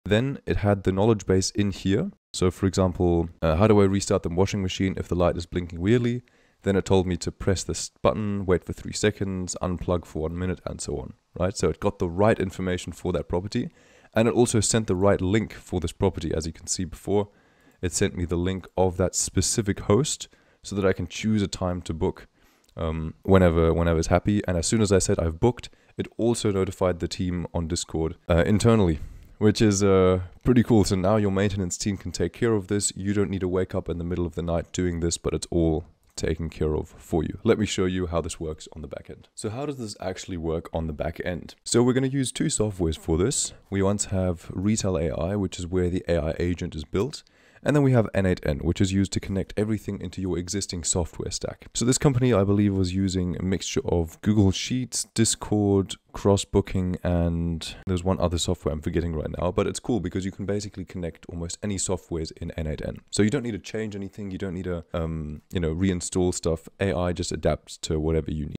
AI Agent Automates Maintenance: Booking & Notifications!
An AI agent can help property managers address maintenance issues. It troubleshoots appliance problems and notifies maintenance teams directly, integrating with tools like Google Sheets and Discord. FULL VIDEO